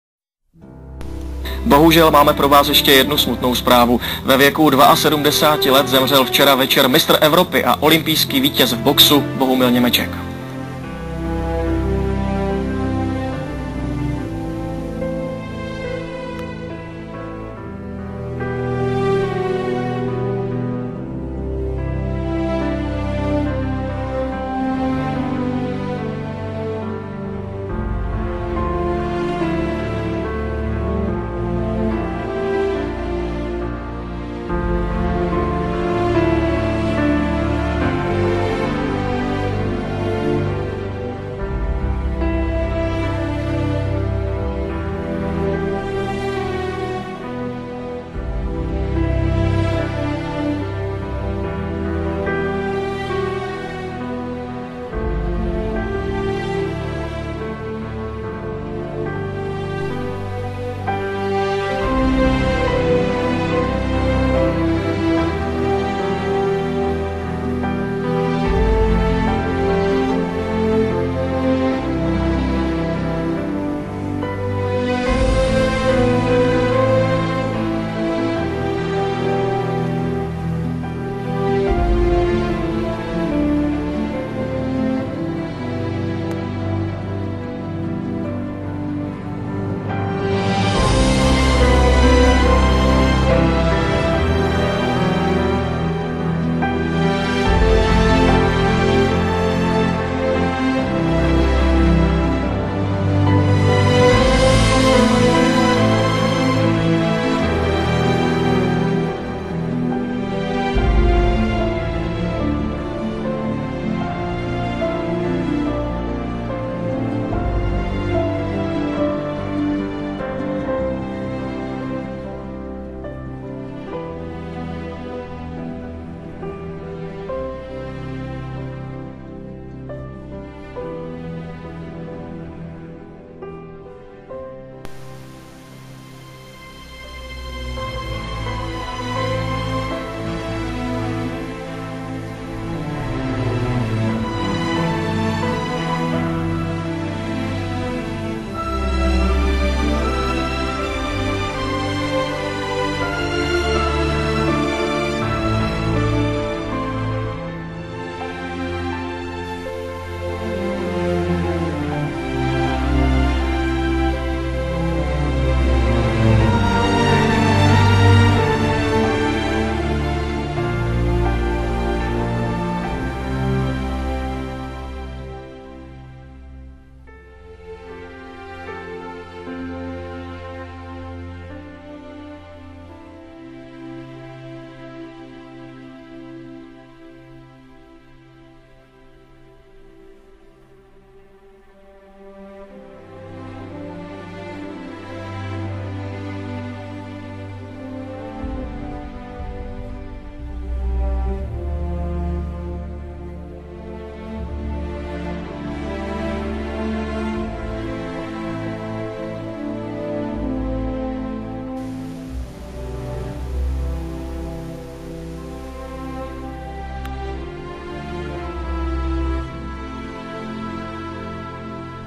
Bohumil Němeček - olympijský vítěz v boxu - Řím 1960
Svého největšího úspěchu dosáhl Němeček v roce 1960 na XVII. olympijských hrách v Římě, kde vybojoval zlatou olympijskou medaili v jedné z nejlépe obsazených váhových kategorií v lehkém velteru do 63,5 kg. V soubojích nevynikal tvrdými údery, ale vytříbenou technikou a „rychlýma nohama. Postupně vyřadil největší favority Itala Pietra Brandiho a Američana Quincey Danielse, aby ve finále s Williamem Quarteyem z Ghany dominoval ringu. Na mistrovství Evropy 1963 v Moskvě startoval už ve velterové váze do 67 kg a třetím místem dokázal, že je stálicí světového ringu. S nadějemi odjížděl i na XVIII. olympijské hry 1964 v Tokiu, ovšem ve druhém kole ho vyřadilo typické boxerské zranění ‑ prasklé obočí. Hned v prvním kole opustil olympijský ring také na dalších olympijských hrách v roce 1968 v Mexiku. Mezitím mu však „věčné město přineslo opětovné vítězství. Na mistrovství Evropy v roce 1967 porazil ve velterové váze všech jedenáct soupeřů a zaslouženě se stal evropským šampiónem.